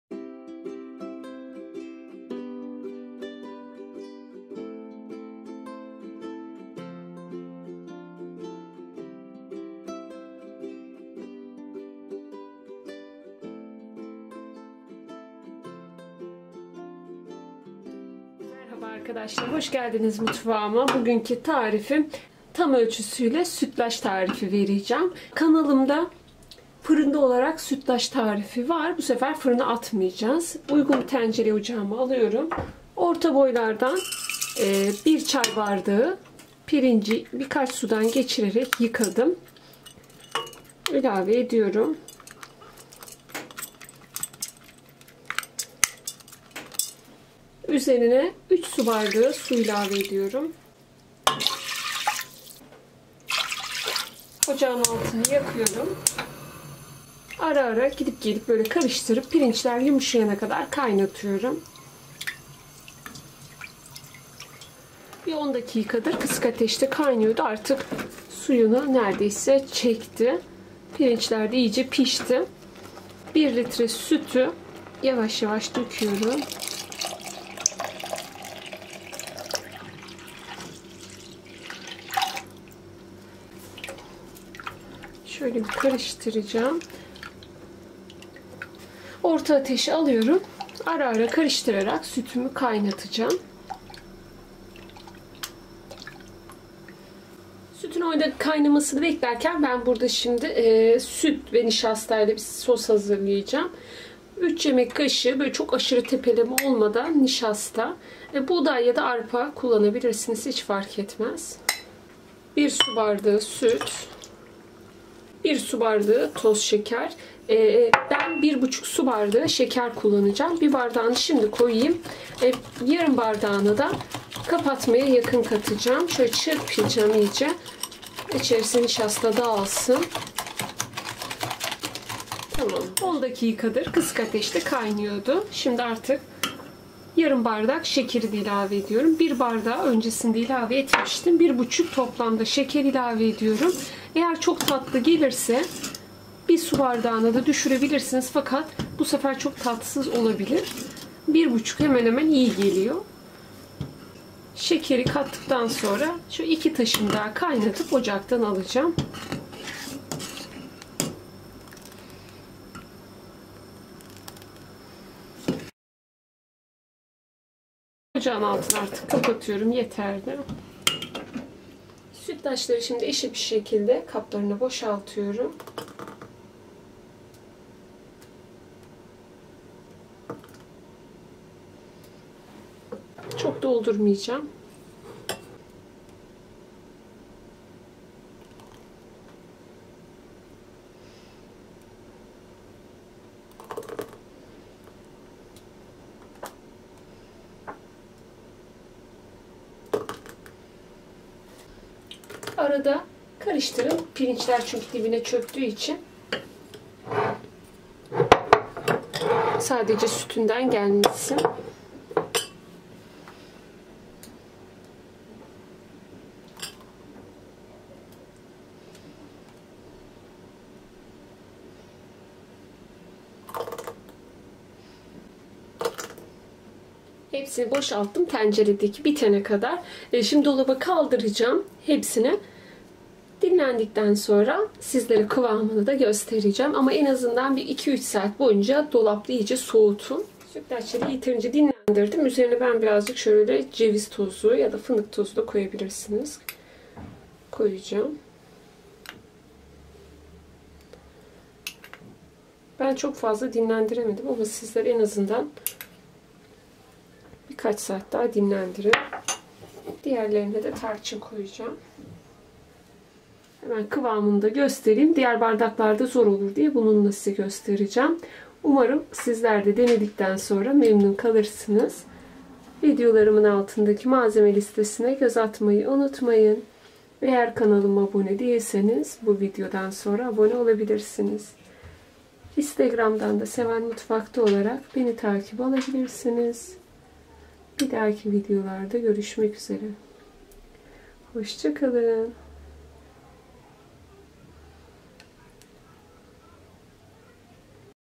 Herkesin Severek Yediği Tam Ölçülü Sütlaç Tarifim💯İşte Aradığım Sütlaç Tarifi Dedirtir/Seval Mutfak
merhaba arkadaşlar bugün sizlere herkesin severek yedigi tarife bire bir uyarsanız sizlerinde en favori sütlaç tarifi olacağından eminim 👍
❗️video içinde nişasta karışımını eklerken kamerayı kapatmışım kusuruma bakmayın lütfen bazen böyle aksilikler olabiliyor 🙏 sizlerde pirinç ve süt kaşımını 10 dakika kaynattıktan sonra karışımı yavaş yavaş ilave edip arar ara karıştırarak 10 dakika daha kaynatıp kapatın 👍
umarım sizlerde denedikten sonra çok daha güzel sonuçlar almanız dileğiyle hayırlı bereketli günler diliyorum 🙏❤️🌺

🍃malzemeler
1 litre süt
1 çay bardağı pirinç
3 yemek kaşığı nişasta
1 ,buçuk su bardağı şeker
3 su bardağı su(pirinçi haşlamak için)
1 su bardağı süt (nişastayı açmak için)

üzerine isteğe bağlı olarak:ceviz yada fındık tozu